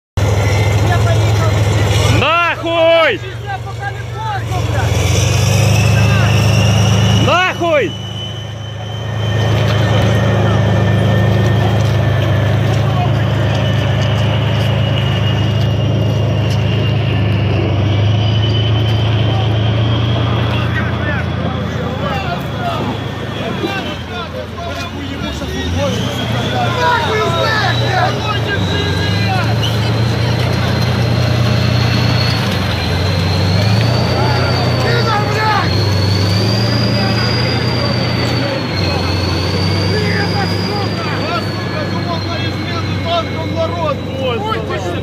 Ukranian citizens stopped Russian tanks from entering Ukraine
More Video of unarmed Ukranian citizens blocking roads and forcing Russian tanks to turn around.